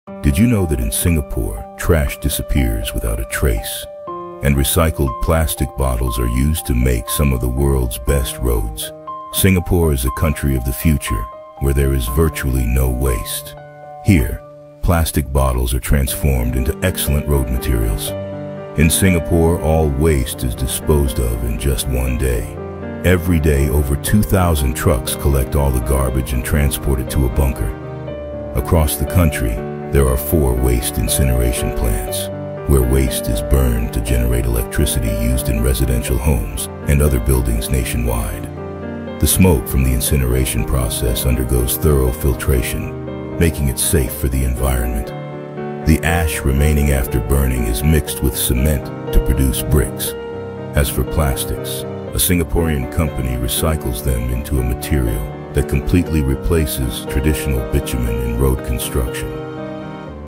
The country where trash disappears…
In Singapore, waste doesn’t pile up — it powers homes, builds roads, and disappears without a trace. With high-tech incineration, advanced recycling, and zero-waste innovation, even plastic bottles become road material.